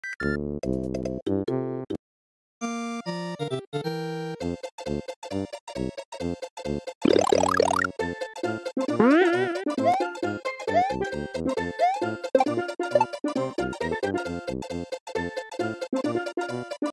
C3 Winter 2019 Request 1: Pipe Jumping Rex
A Rex that can be placed in a pipe or any surface amd make it look at various sides, that when Mario gets closer, it jumps like a surprise attack and follows you.

Requested by Blizzard Buffalo